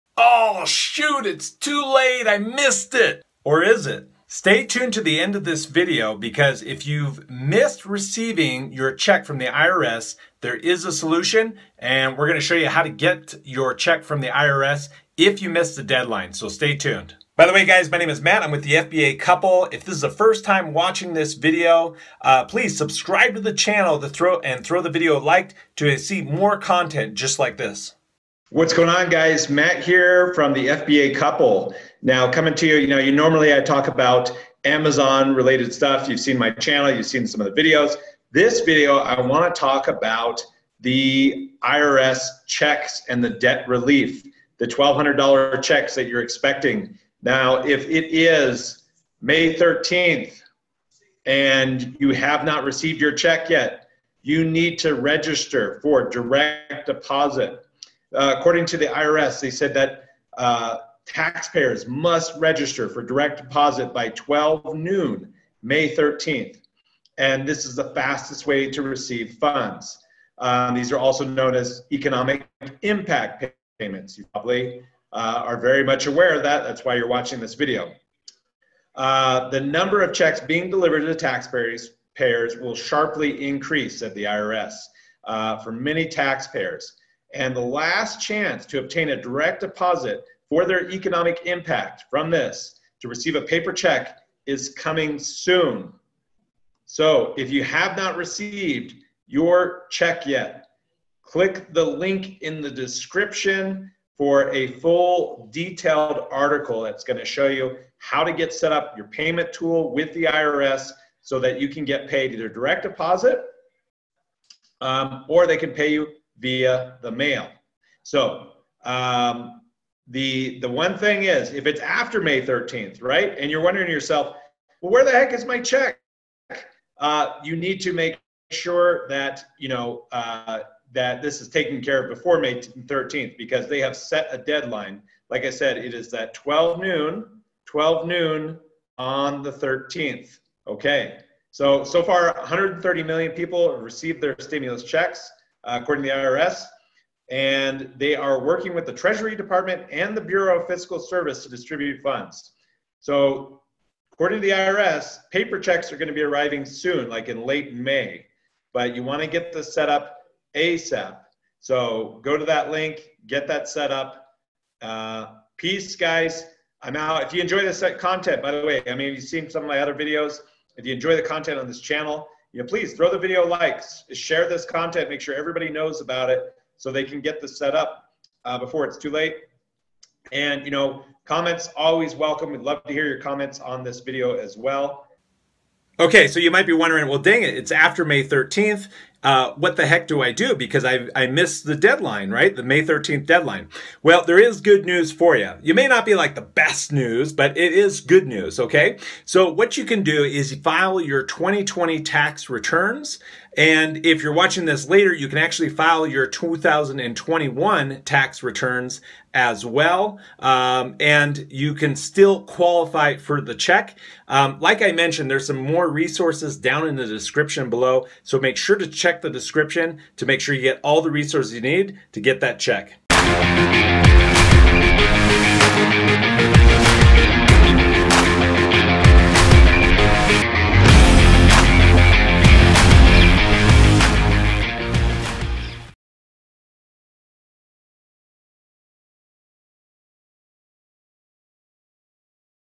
Didn't Get [$1200 IRS Stimulus Check]. What Now?
Didn't get your stimulus check? As long as you qualify for the irs stimulus check you may still receive money. As long as you qualify but you failed to sign up with the IRS for direct deposit then you will receive payment via mail. Even if you missed the May 13th Stimulus Check Deadline.

Instructions on how to get your stimulus check if you missed the deadline: